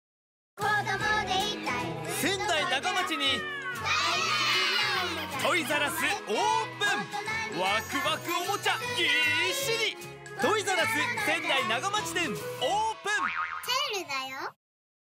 トイザらス 仙台長町店 10/19(木) グランドオープン！
わくわくオモチャぎ～っしり！
グランドオープン記念にセール、イベント、プレゼントを開催！
※プレゼントはなくなり次第終了

さらに、トイザらス・ベビーザらス 名取りんくうタウン店でも合同セールを開催！